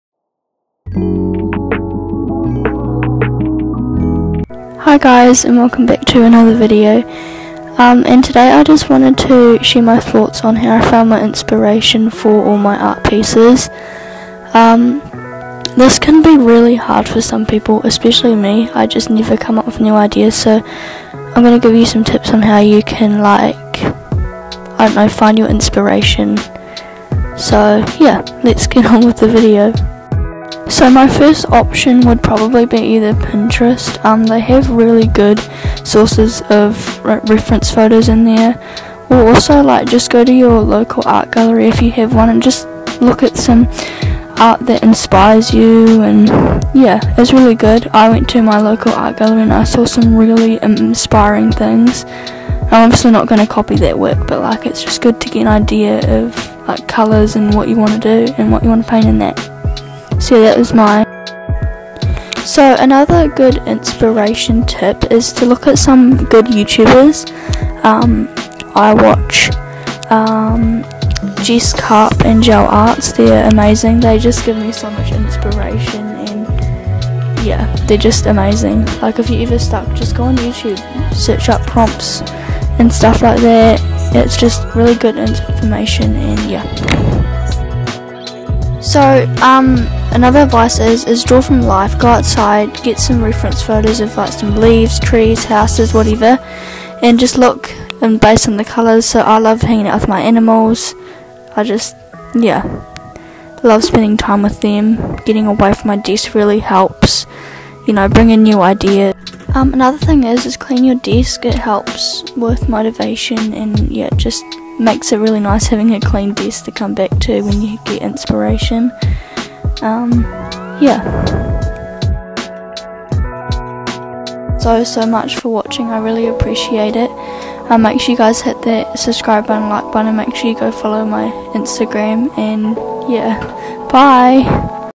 How i find my inspiration for my art
Hi there this video is about finding inspiration some times it can be really hard for in artist to find inspiration. So thought I would make a video of sharing my Personal thoughts that have helped me get inspiration. Hope you guys enjoy.

Filming: iPhone XR ￼